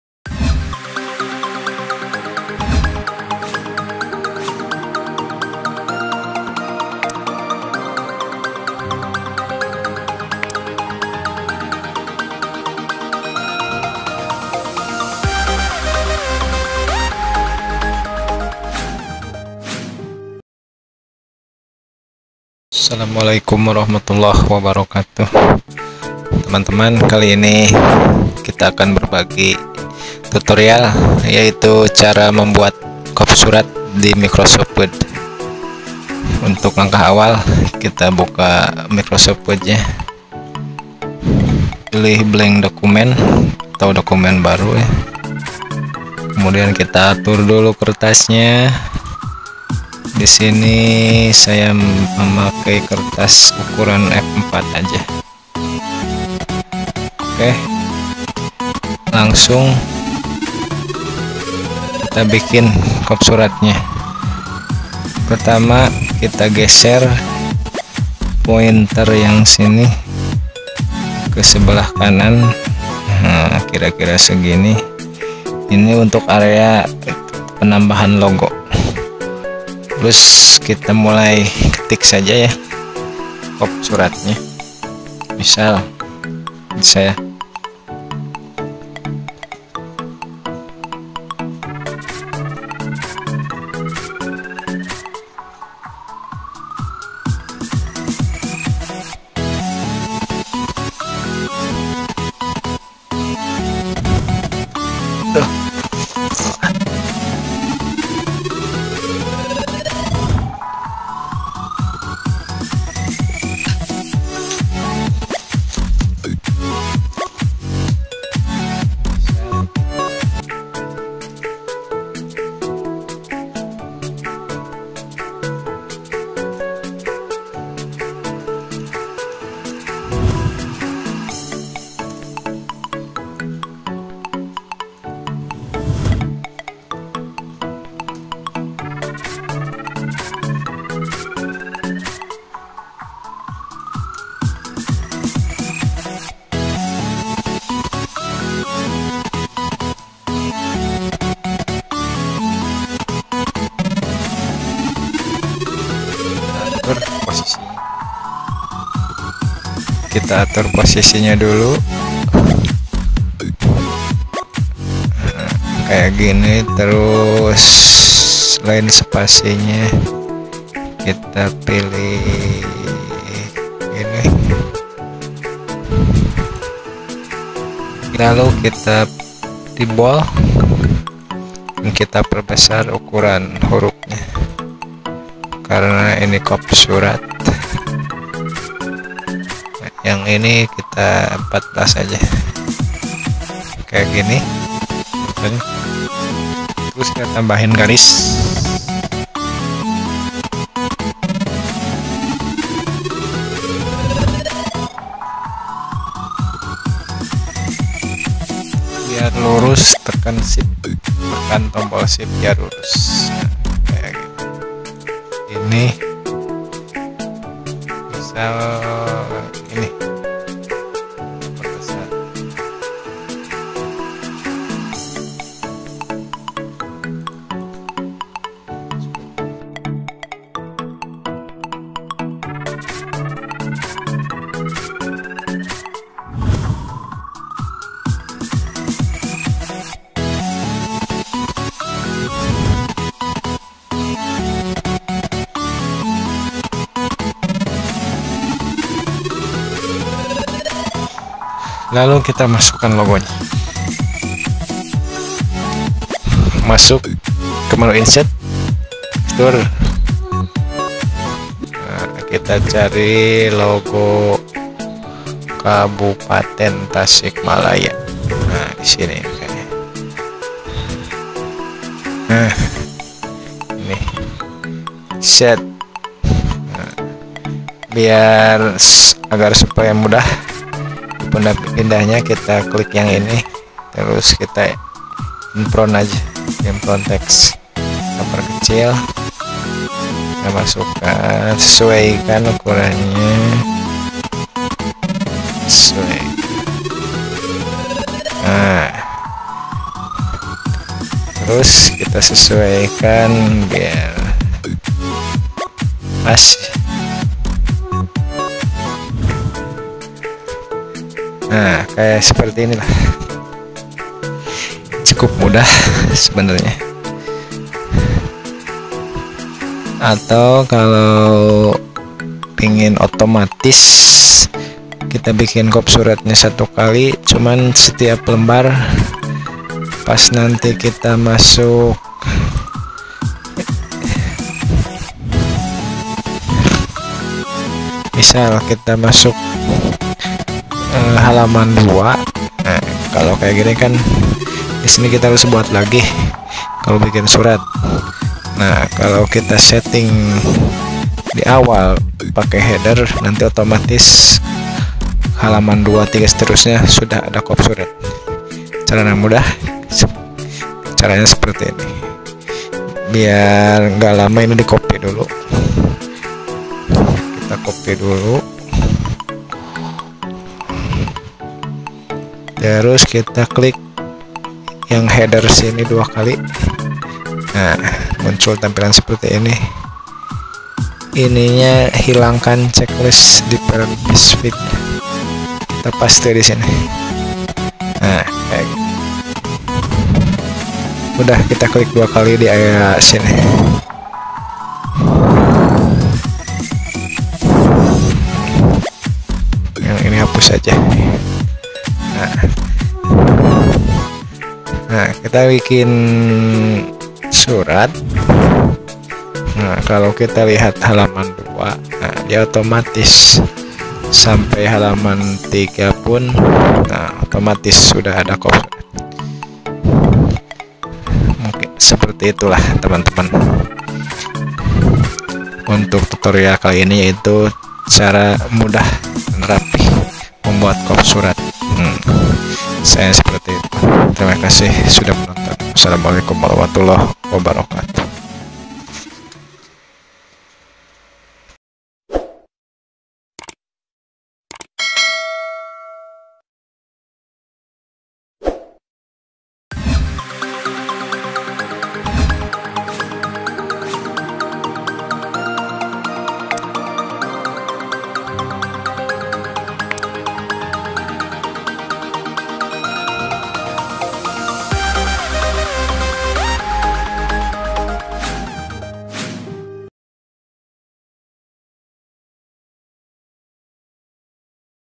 Cara Mudah Membuat KOP Surat di Word dengan Logo dan Garis | Tutorial Word
ini adalah Video Tutorial Word cara membuat Kop Surat dengan mudah dan Rapih dengan menyertakan Logo dan garis , serta membuat kop surat otomatis di setiap halaman kerja.
semoga tutorial ini dapat bermanfaat bagi teman-temen pemula yang baru mengenal ms. word, sehingga pekerjaan kita semakin cepat selesai.
Terima kasih.

Salam Hormat